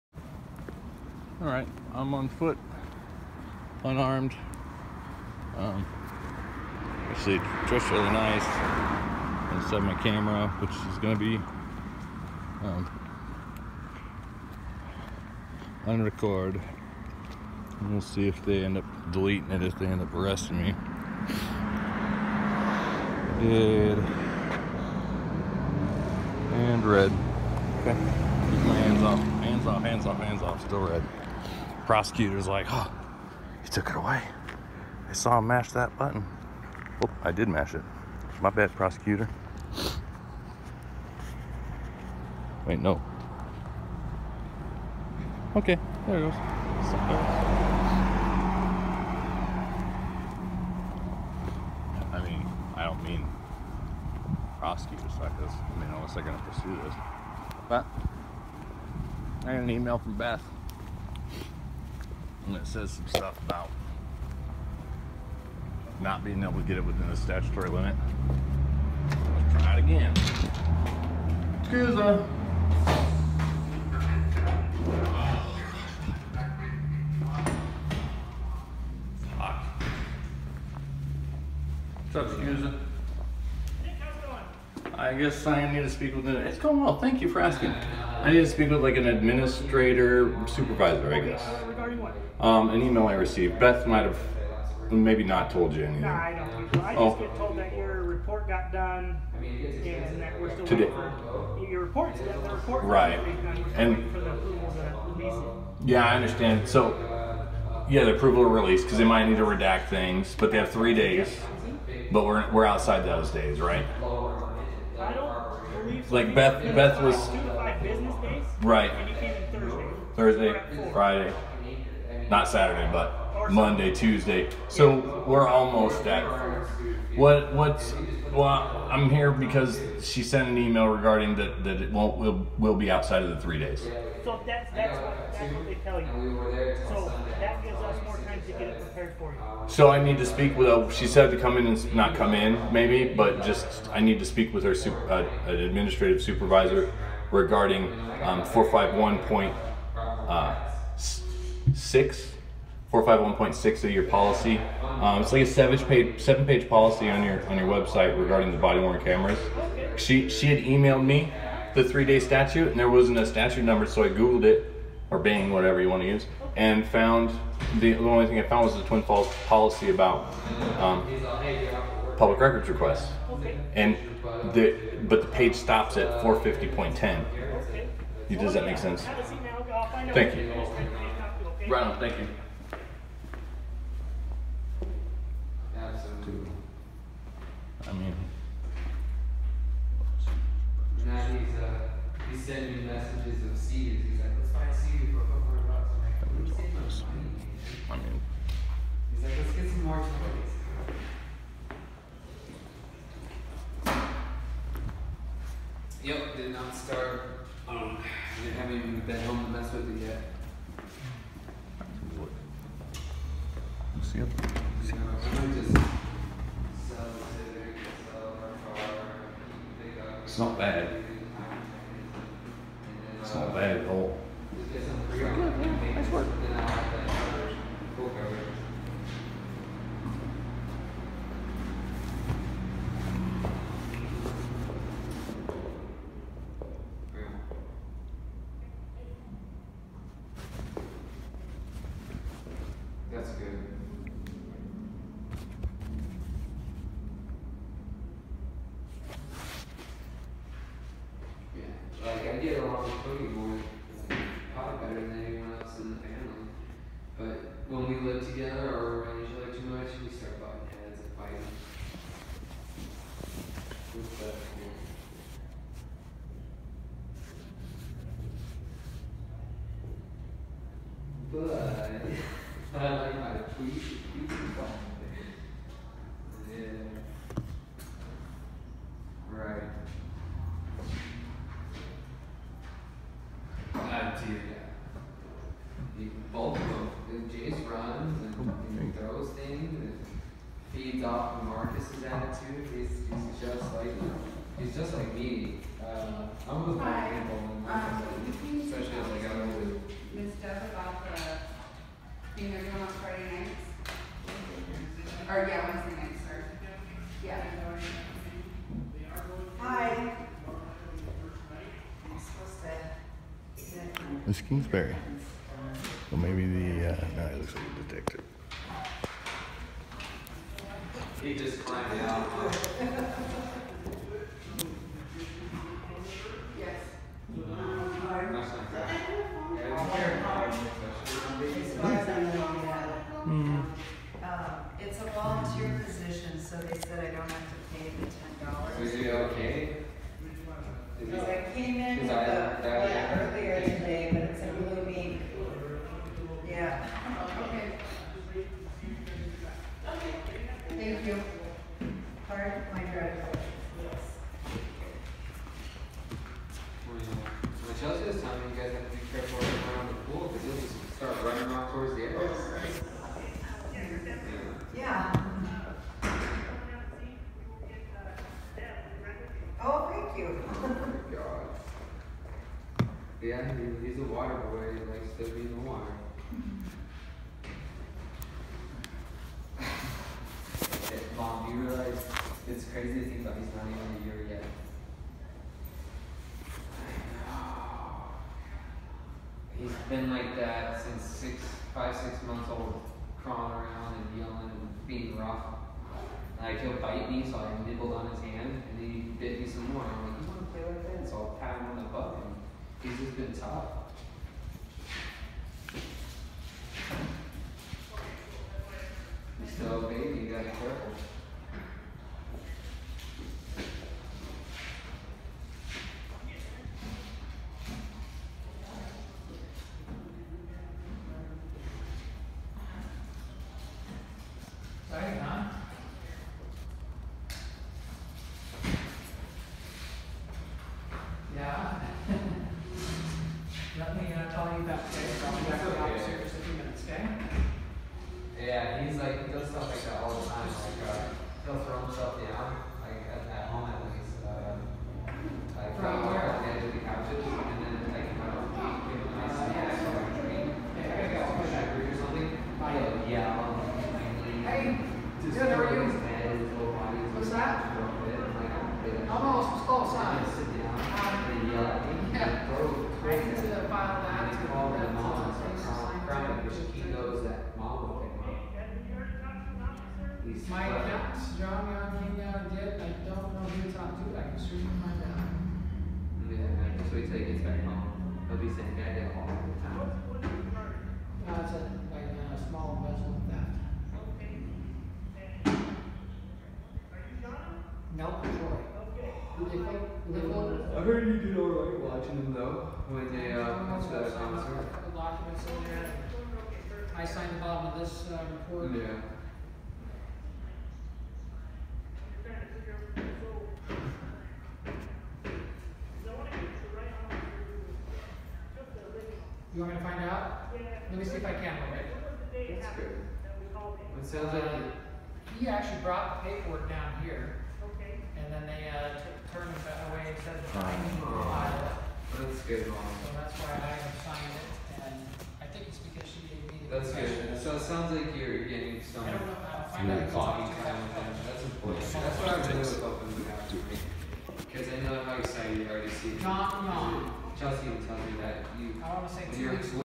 Audit: Public Records Request
I received an email about speaking to a supervisor about 251.6(B) and a delay in my request.